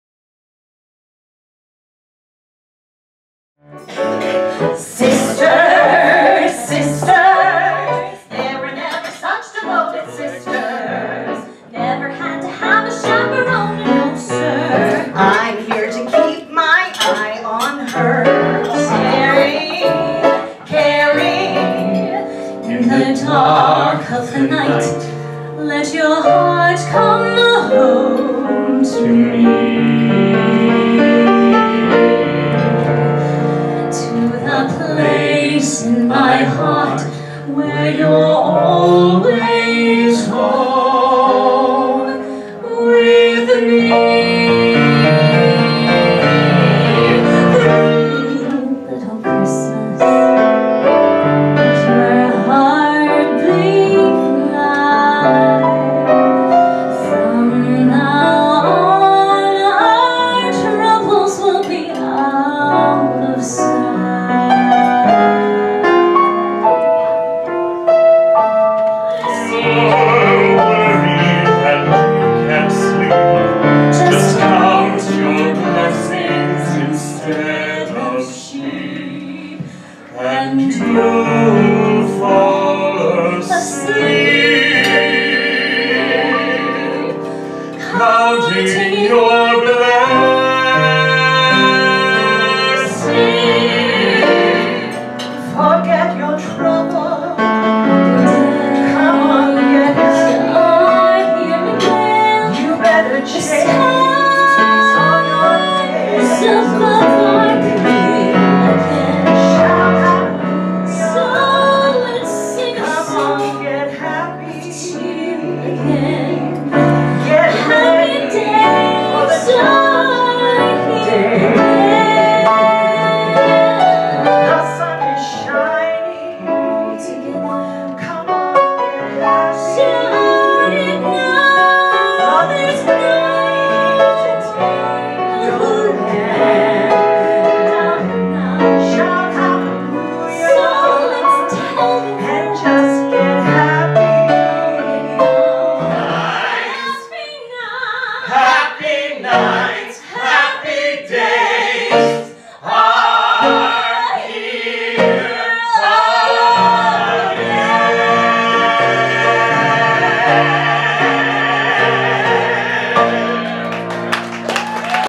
Christmas at Nighttown
Kathleen Rooney Faubel sings at NIGHTTOWN Jazz Club in Cleveland, OH with Shane O'Neill, Robert Pierce, and Natalie Blalock